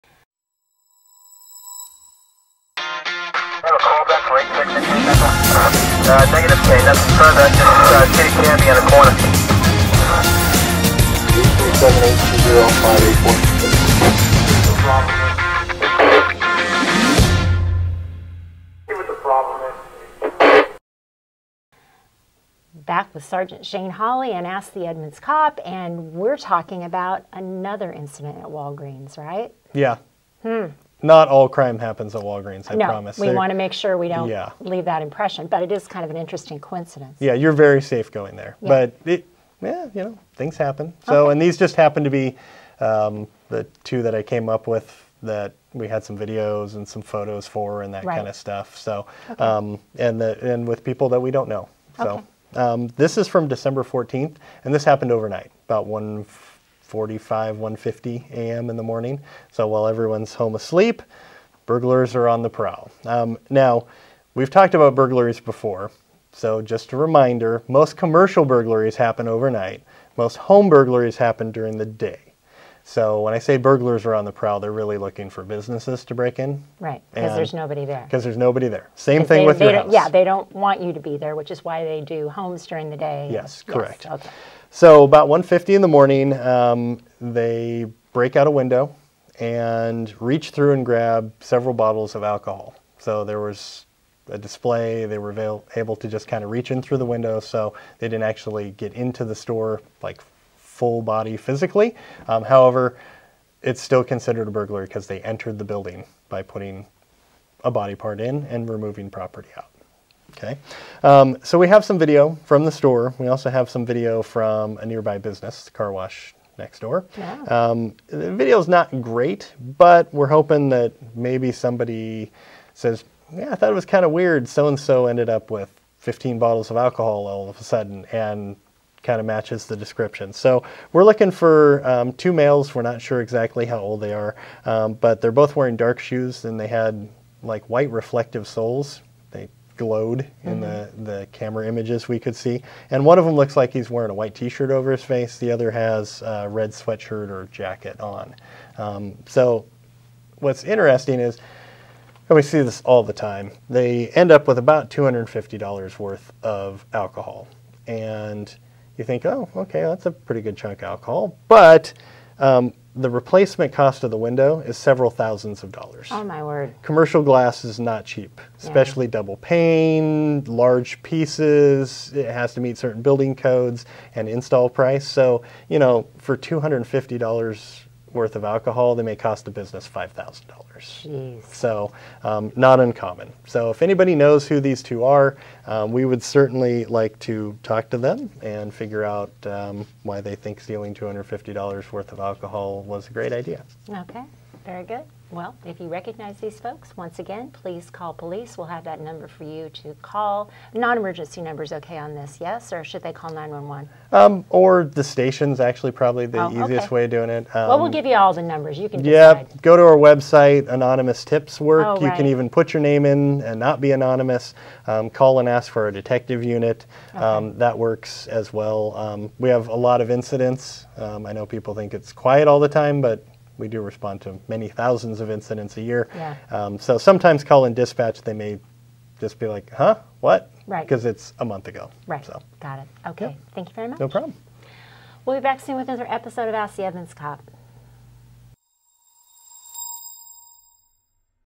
Ask the Edmonds Cop: Do you recognize these burglary suspects who stole alcohol from Walgreens?
Edmonds police are searching for suspects who broke a window and stole alcohol from a display at the Walgreens on Edmonds Way. Or online tips can be submitted through